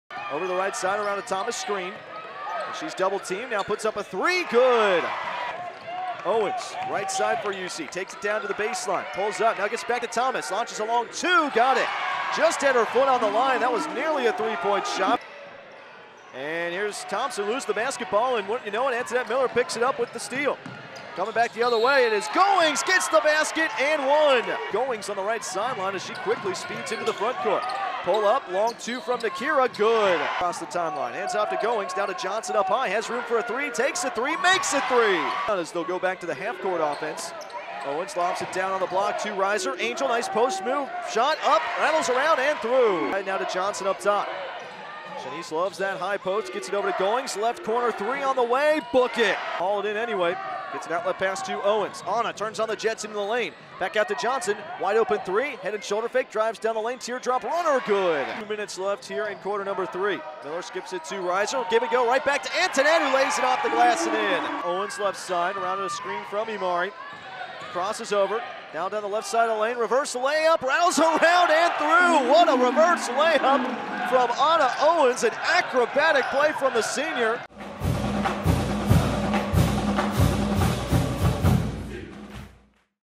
Women's Basketball Highlights: Cincinnati 65, Southeast Missouri State 52
The Bearcats defeat the Southeast Missouri State Redhawks 65-52 on Thursday night. Johnson led all scorers with 19 points and 10 rebounds.